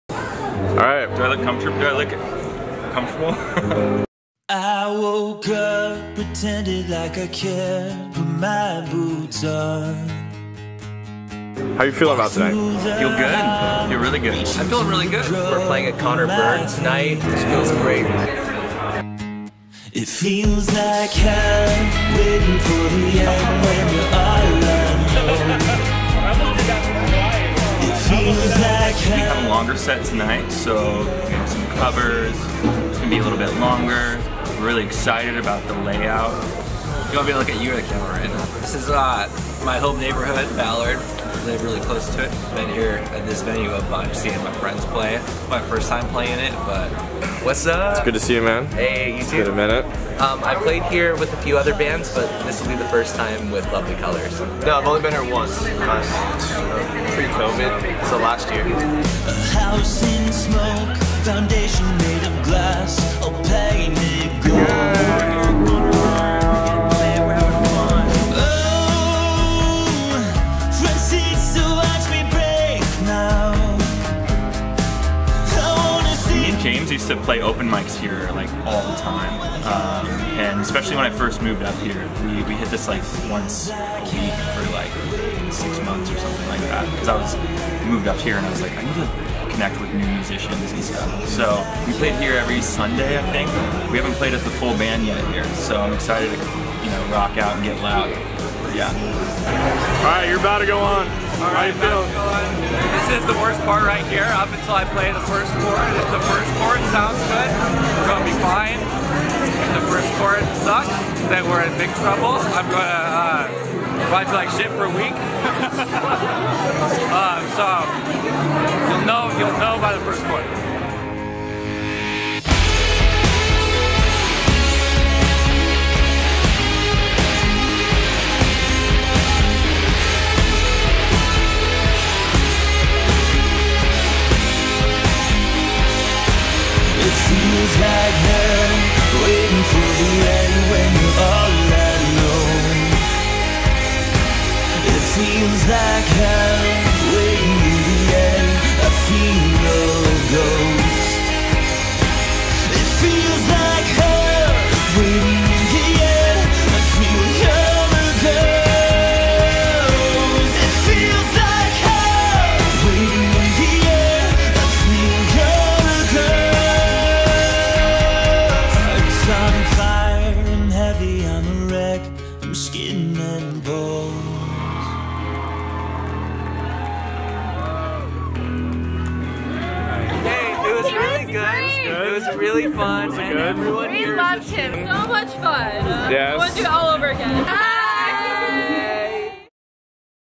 LCTV - Episode 1
Watch the boys as they gear up for their show at Conor Byrne!

Directed by Nick Janning